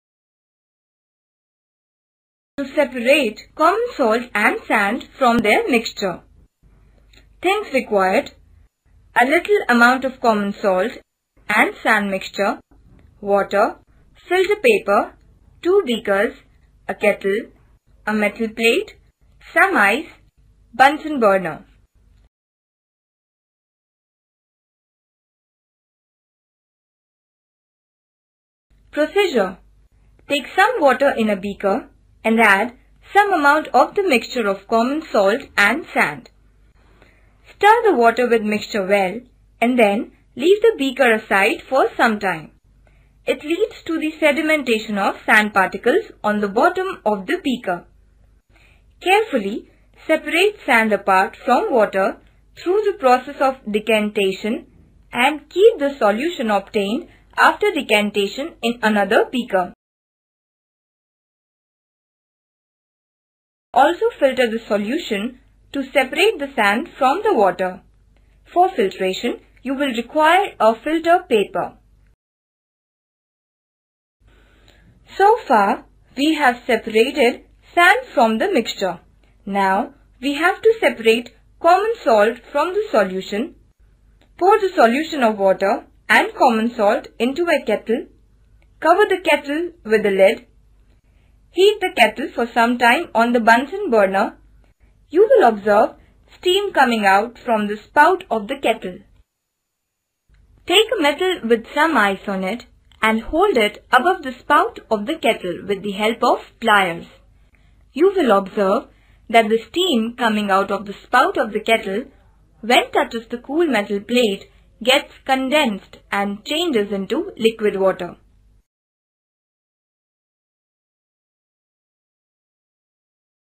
separate sand and salt mixture cách tách muối và cát ra khỏi hỗn hợp
Dựa vào tính chất vật lý: cát không tan trong nước, muối tan trong nước